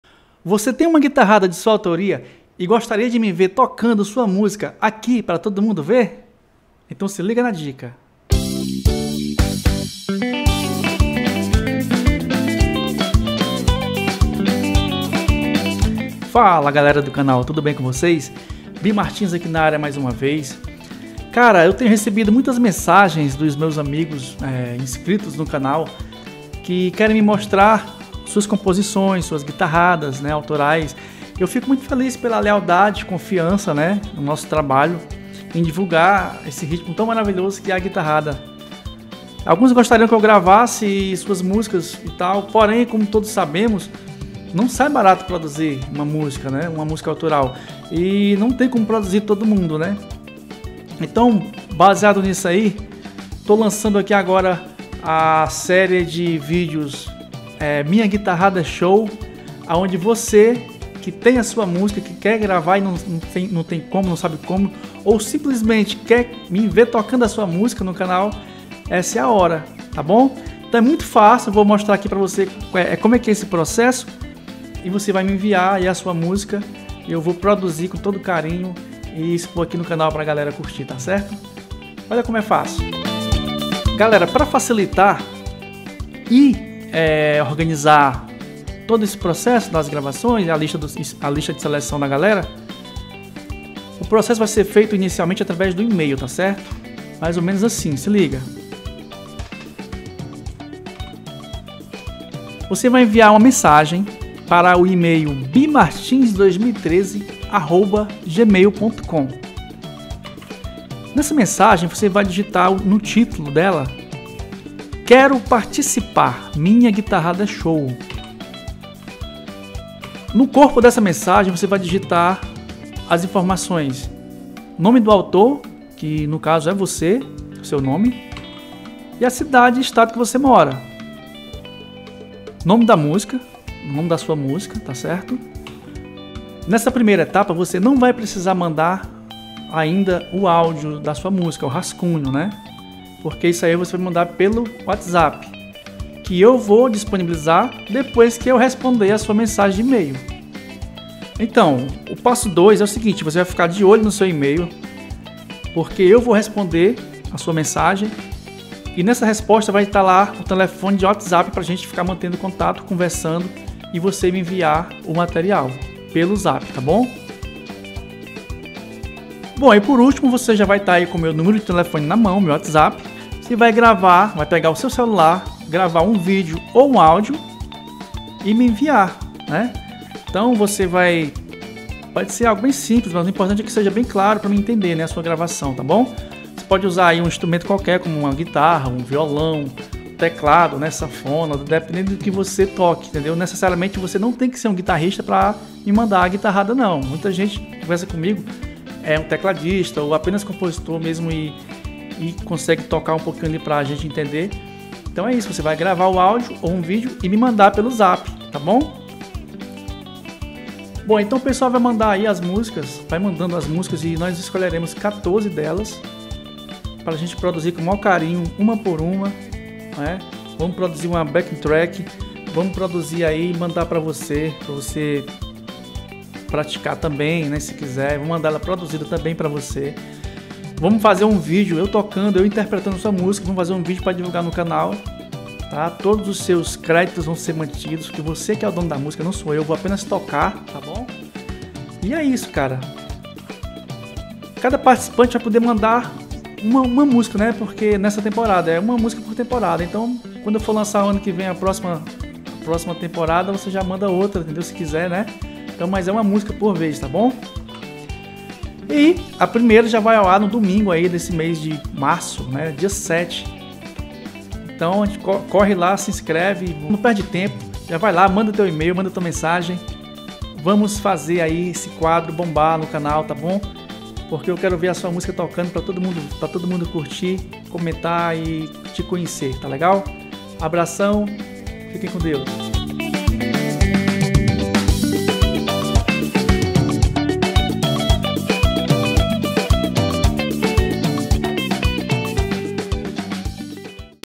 VOCÊ COMPÕE? PARTICIPE DA SÉRIE "MINHA GUITARRADA É SHOW" | LANÇAMENTO OFICIAL
Hoje estou aqui lançando essa novidade super bacana pra você que é músico amador ou mesmo profissional, tem suas composições, mas não tem como produzi-las ou gravar ou simplesmente gostaria que eu tocasse sua musica no canal.
A ideia é simples: Você vai se inscrever e mandar o rascunho da sua guitarrada e ela será produzida e gravada por mim, Bimartins,  com maior carinho e ao final da produção gravaremos um   vídeo aonde eu vou interpretar sua música, em sua homenagem no canal, com todo o seu créditos de autor/compositor mantidos. ok?

Para participar da primeira temporada é fácil:

Assunto/titulo:  "Quero participar - Minha guitarrada é show"
- autor/compositor:
- cidade/estado:

2- Você receberá um e-mail de resposta contendo um numero de WathsApp, grave um rascunho, (pode ser um áudio ou um vídeo, direto do seu celular)  demonstrando seu solo e melodia e envie.
PS- Você pode usar qualquer instrumento pra isso, violão, guitarra, teclado, sanfona, etc, o importante é que transmita de forma simples e clara seu solo e melodia. envie para o numero de ZAP que vc recebeu no e-mail.

3- 14 musicas serão selecionadas nessa temporada, serão produzidas e publicadas  no canal no decorrer do ano corrente.